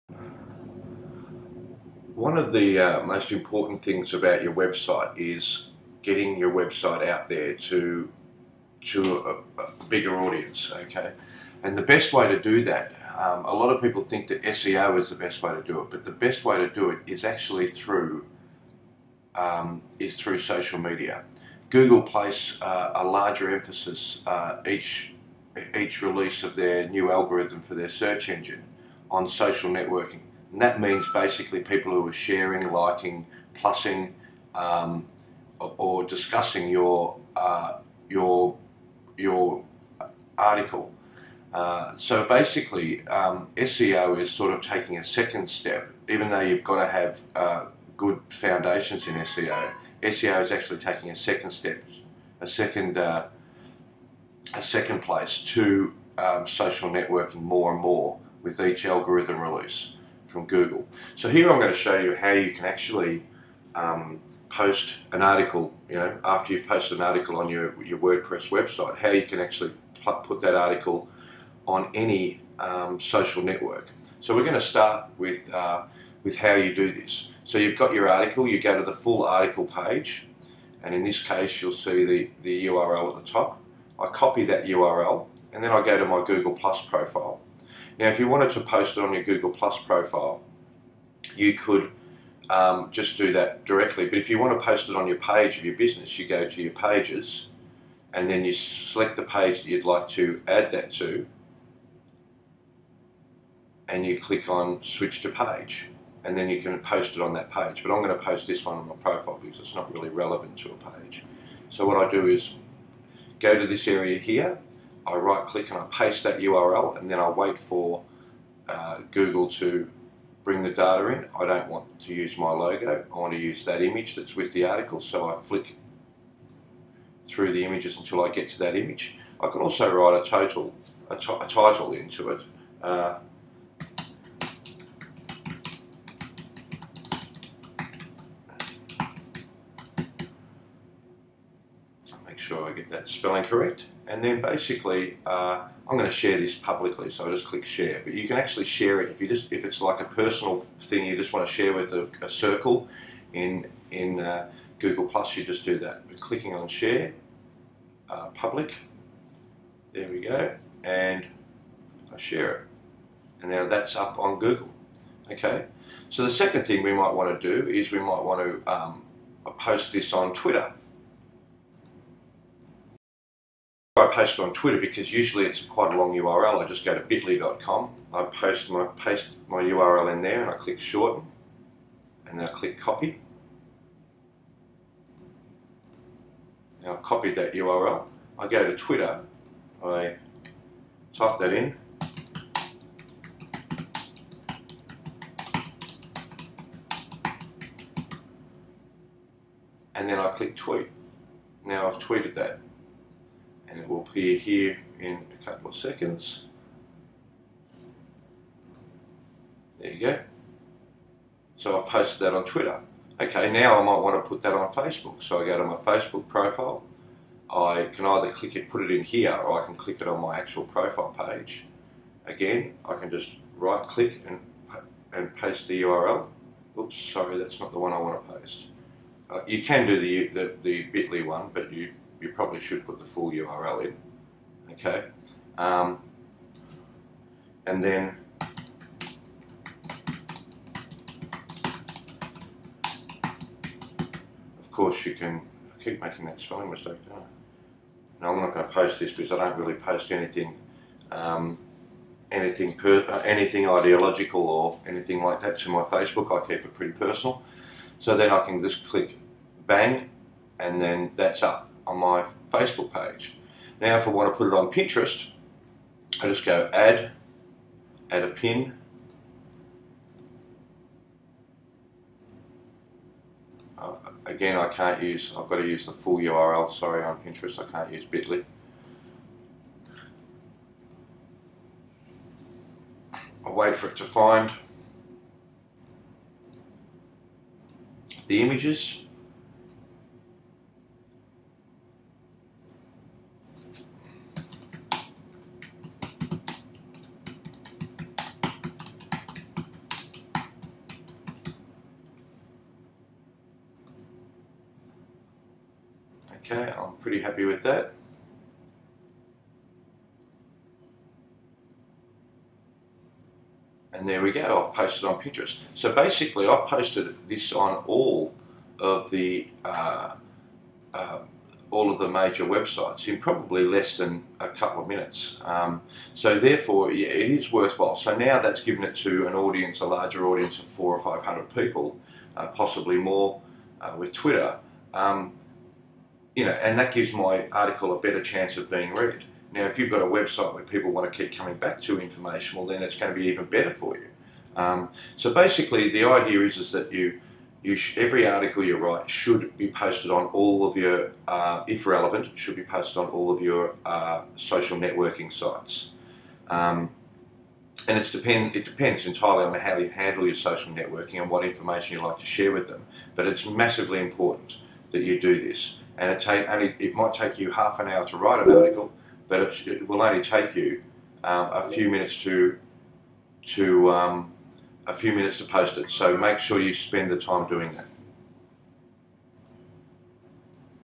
Adding articles to social networks using Wordpress
How to add posts and articles to social networks using Wordpress